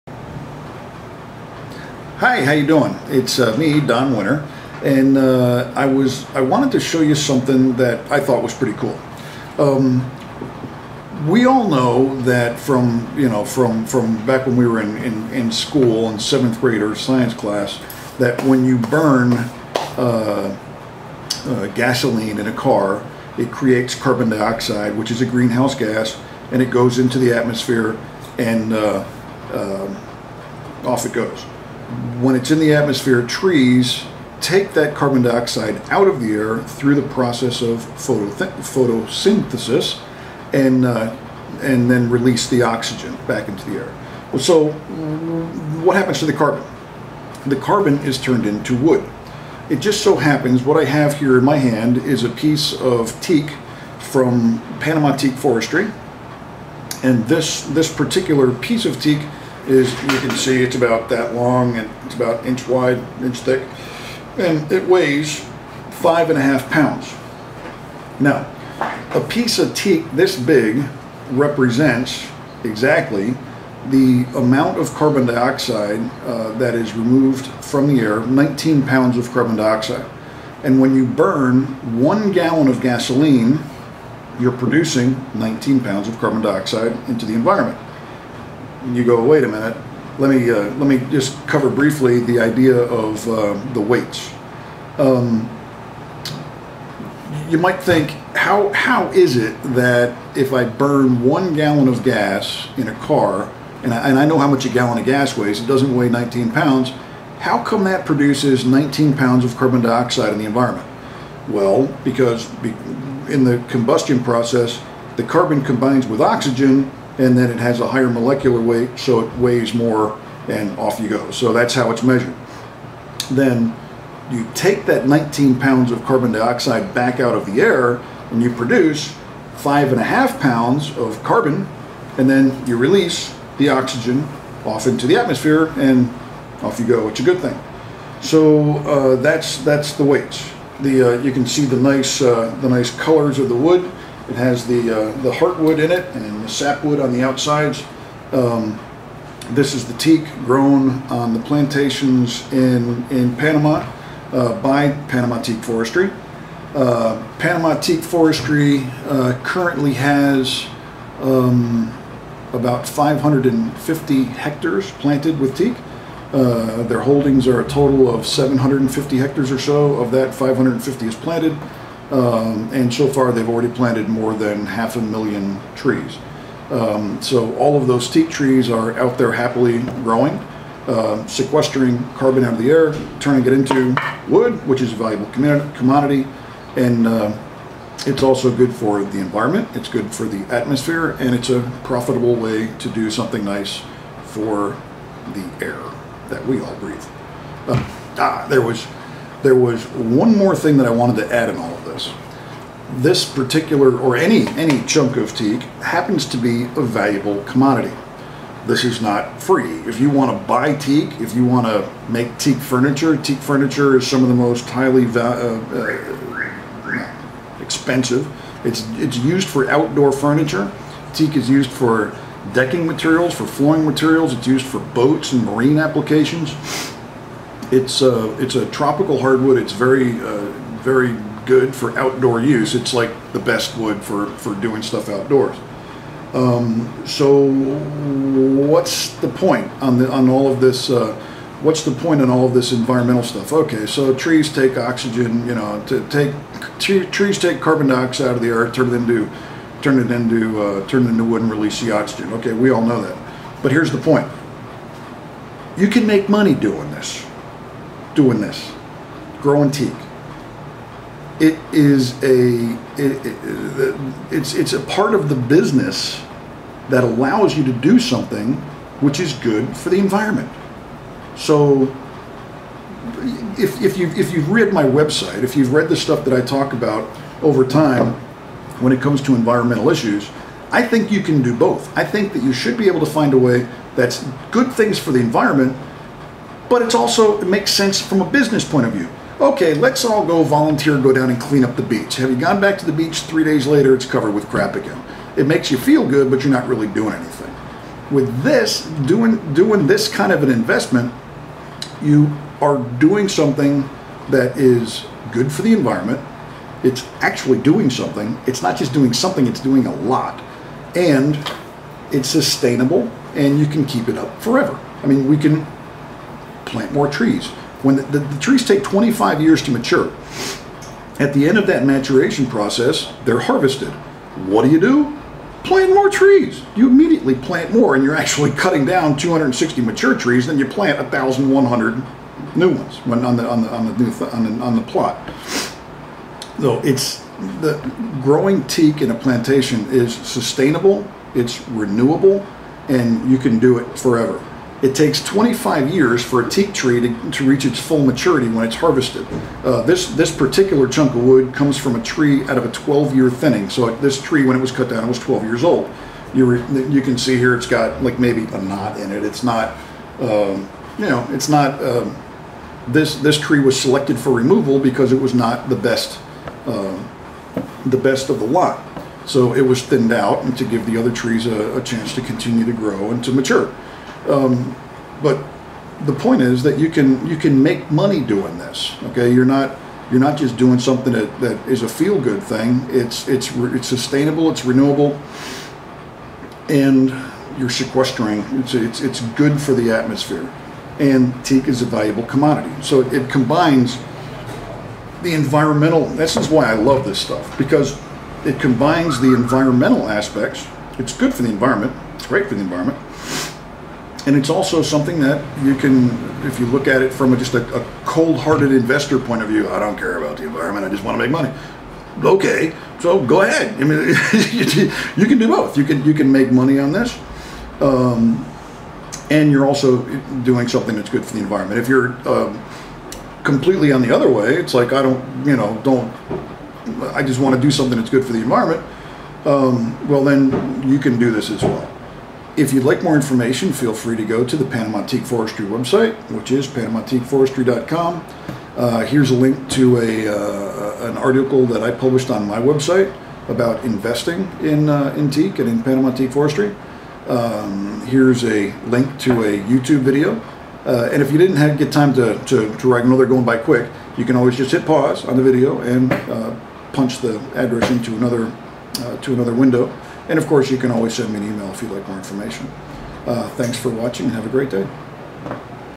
Carbon Sequestration by Teak Trees in Panama
Burning one gallon of gasoline produces 19 pounds of carbon dioxide into the atmosphere. The plantation teak trees of Panama Teak Forestry sequester that carbon and release the oxygen back into the atmosphere. Every 5.5 pounds of teak grown is the equivalent of one gallon of gas burned. Or, said another way, the teak trees sequester 19 pounds of CO2 for every 5.5 pounds of teak wood grown. Panama Teak Forestry has already planted more than a half a million trees...